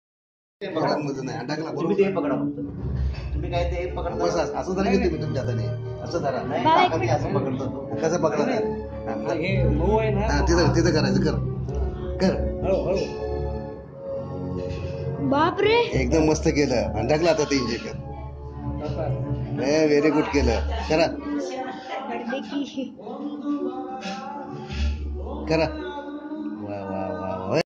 Injection | Diabetes vaccine
Yes, , I m not doctor but my uncle have diabetes so every day he needs injection

Please note : I will continue to share my content, info and support on YouTube.

       adeethbhandarre